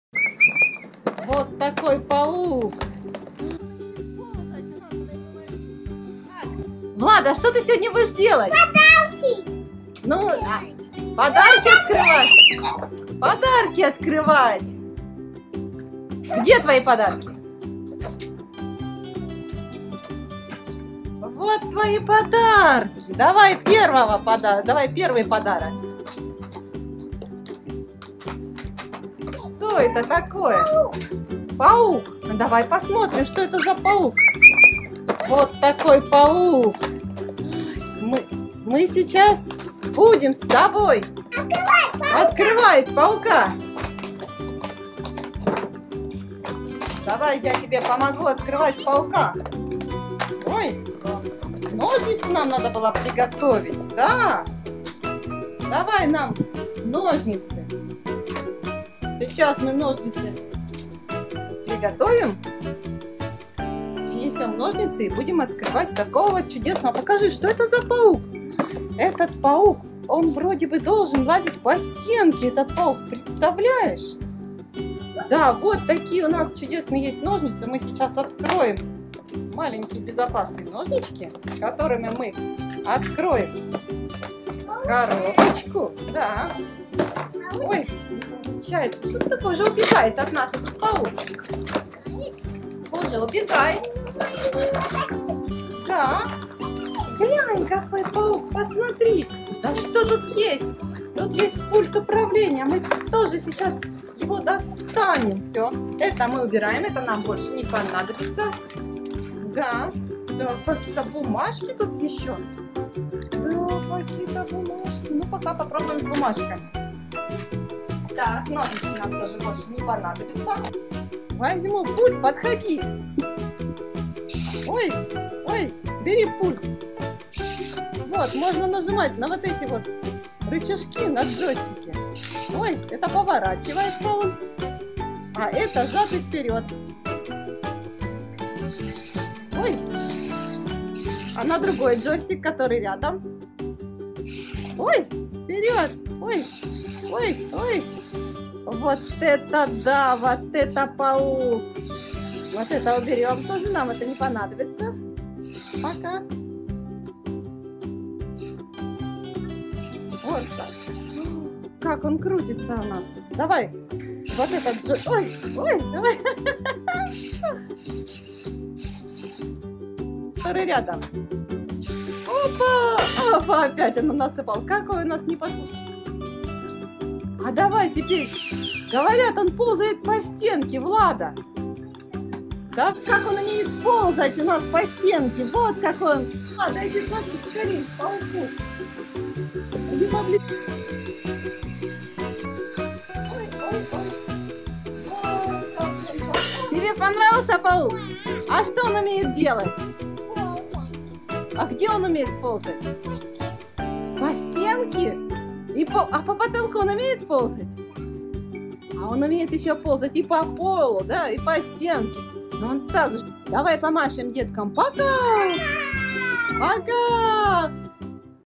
Паук-робот ползает по стенам и потолку. Родители испугались - Видео для детей
Влада управляет новой игрушкой - пауком-роботом , который умеет ползать по стенам и даже потолку

Обещаю, скучно со мной не будет!